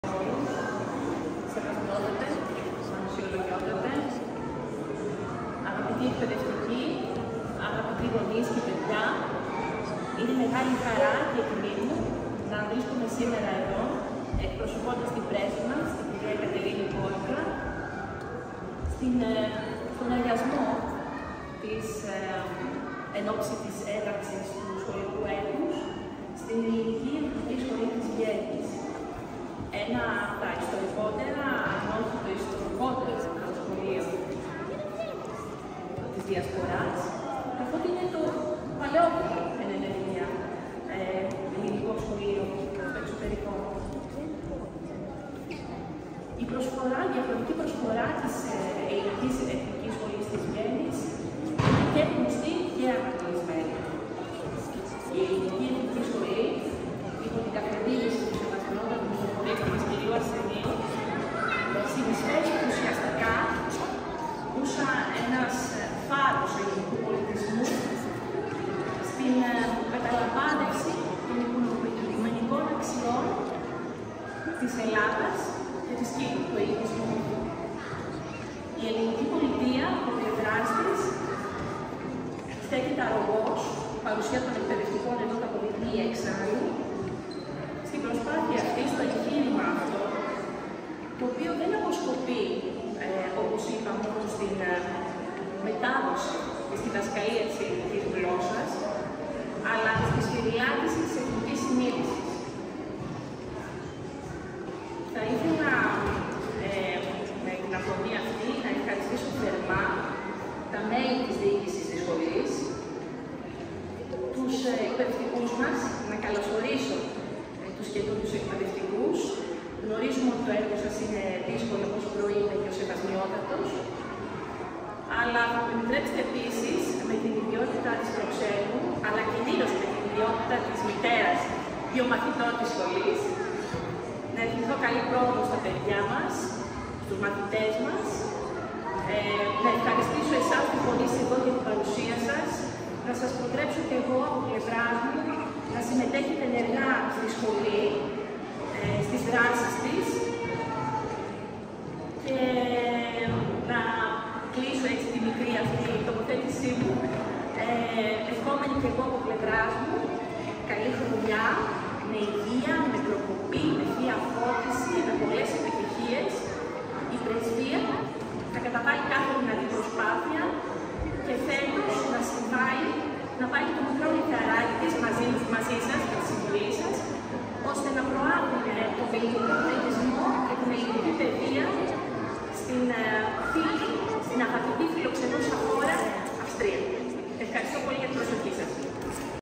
Χαιρετισμός της Προξένου της Ελλάδος στον Αγιασμό της Ελληνικής Εθνικής Σχολής Βιέννης
Χαιρετισμός της Προξένου της Ελλάδος, Ελένης Κυριακοπούλου, κατά την έναρξη της νέας σχολικής χρονιάς στην Ελληνική Εθνική Σχολή Βιέννης. 9/9/23.